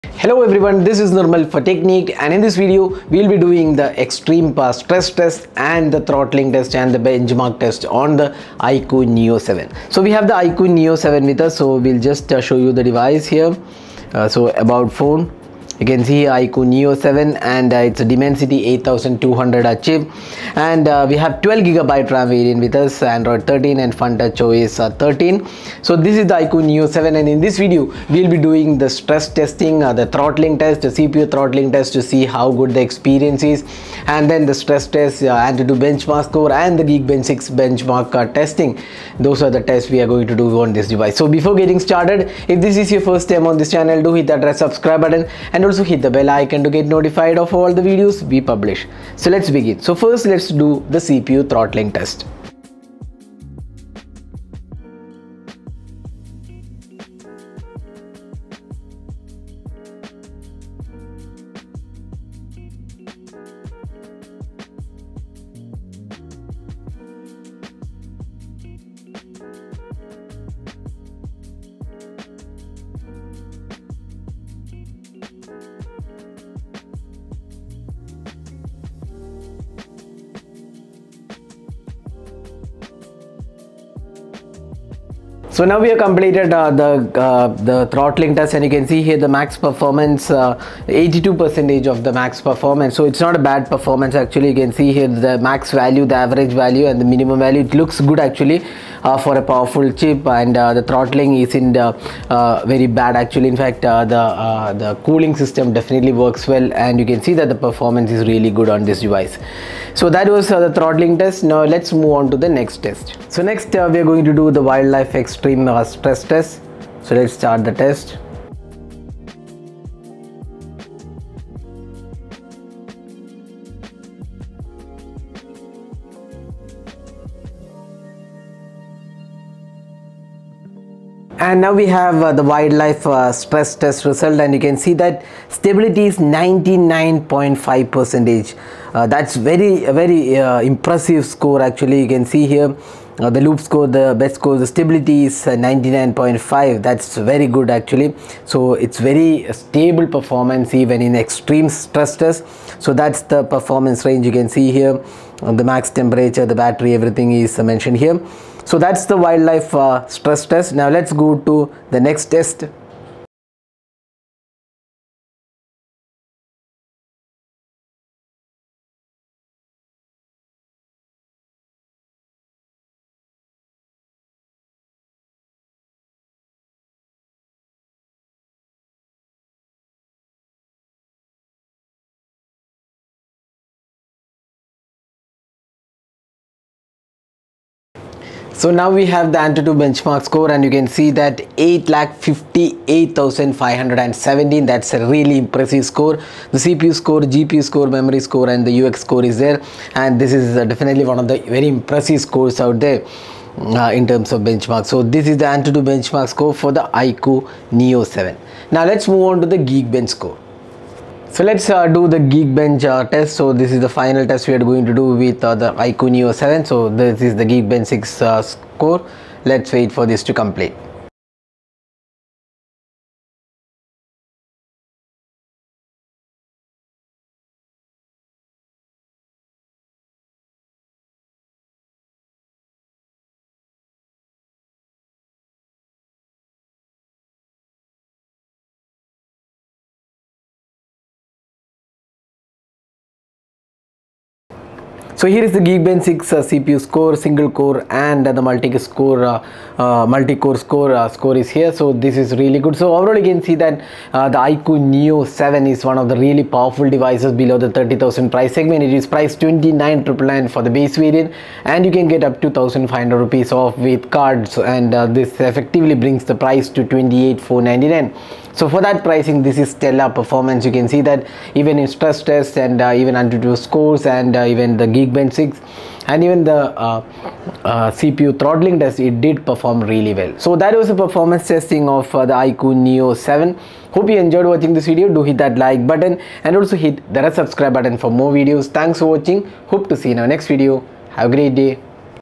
iQOO Neo 7 Performance Test, CPU Throttling, Stress Test, Benchmarks 🔥
iQOO Neo 7 performance test, CPU Throttling test, stress test, benchmark test, and AnTuTu, Geekbench 6 score.